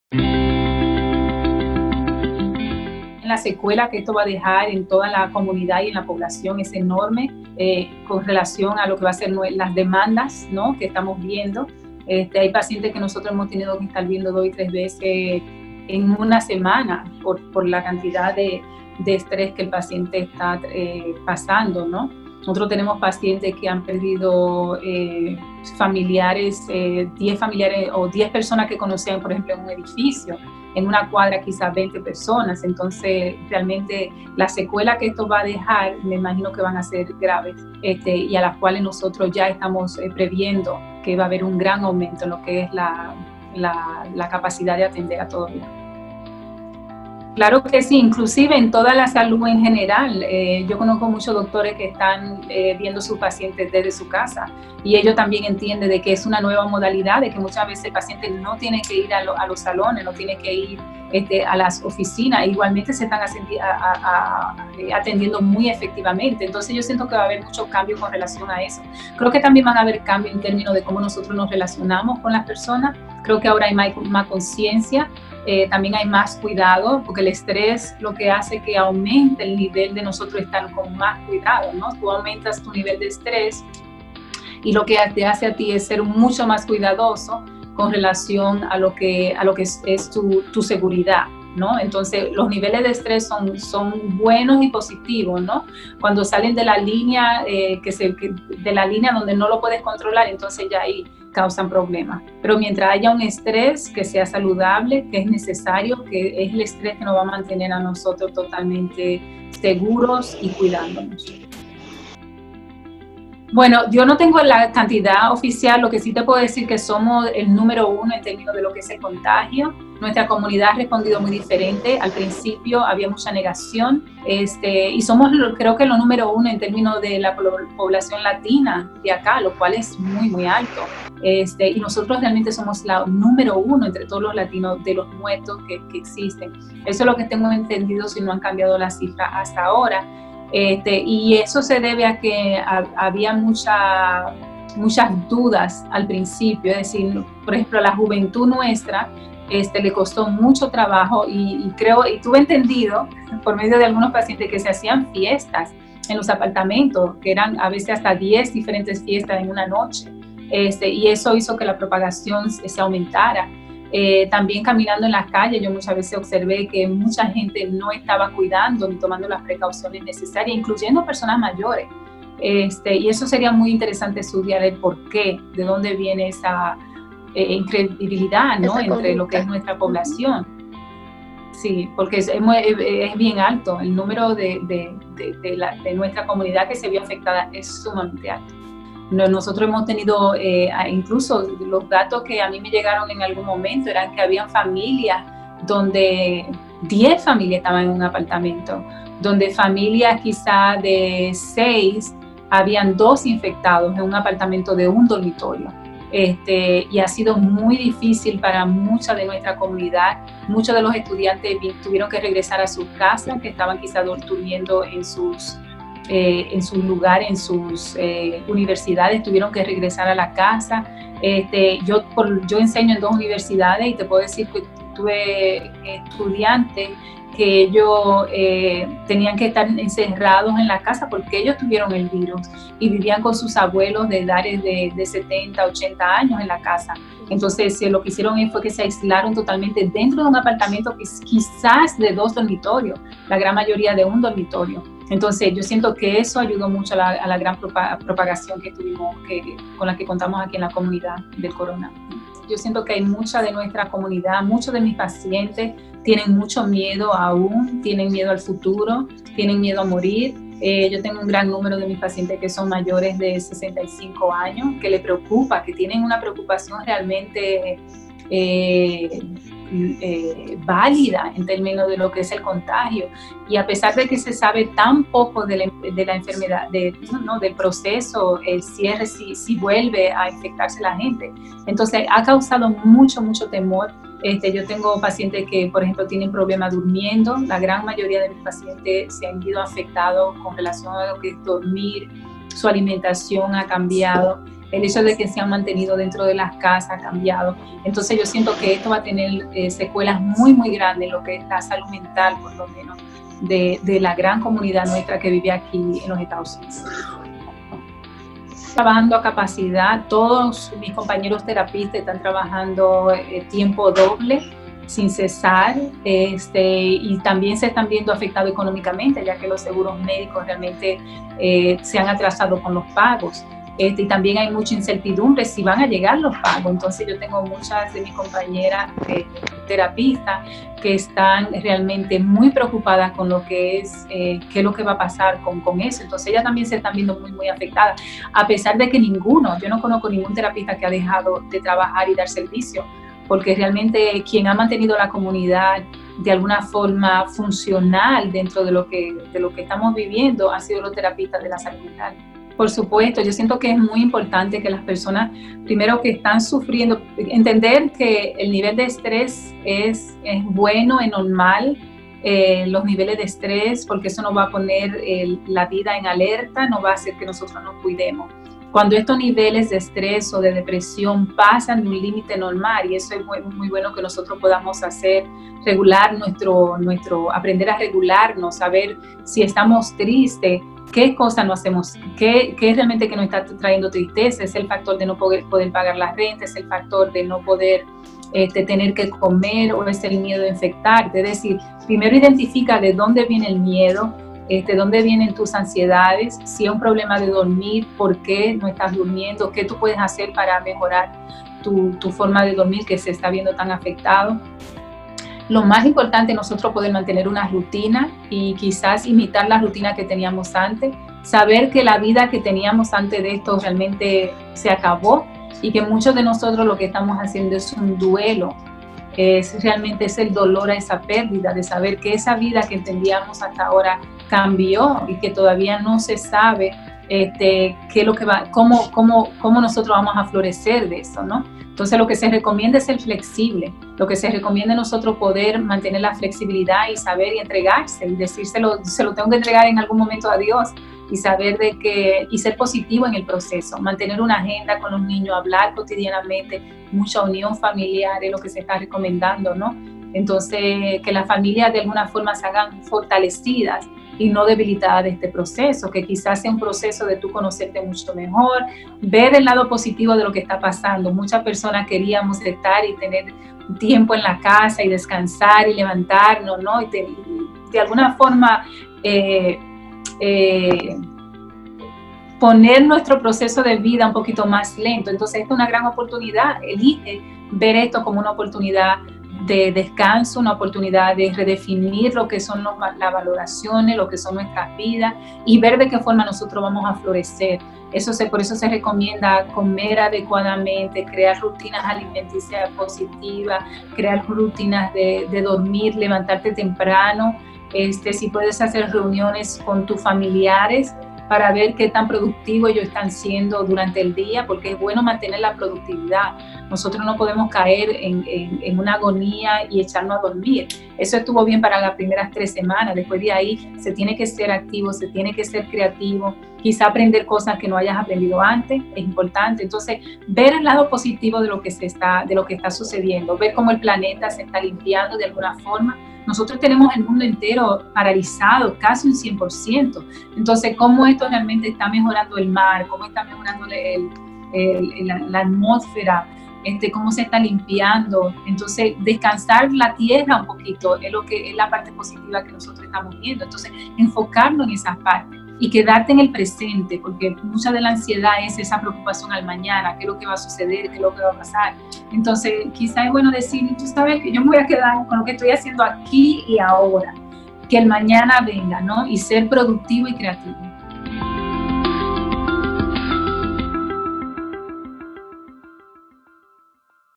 Psicóloga Karina Rieke - Dominicanos en EEUU frente a Covid-19
Los profesionales en el área de la salud, han sufrido una gran estampida de pacientes a causa de la pandemia del Covid-19, pero, los trabajadores en el área de la salud mental han jugado un papel extraordinario en sus pacientes, y es allí donde entra nuestra heroína de hoy, la psicóloga Karina Rieke.

Tras una entrevista realizada por el equipo de investigación de Funglode/GFDD, nos plasmó algunas inquietudes y datos sobre la salud mental en tiempos de pandemia e hizo importantes recomendaciones para el cuidado emocional de todos en medio de este tiempo llenos de caos que nos permitirán tener una mejor rutina y buena salud mental.

Nos comentó que ha observado una gran ansiedad y estrés en las personas que están trabajando directamente con los pacientes enfermos y en los trabajadores de salud mental, aún trabajando desde sus casas, donde menciona que al principio de la pandemia era peor, ya que había muchas quedas debido a que muy pocos disponían de las herramientas de seguridad primaria que se necesitan para protegerse del virus.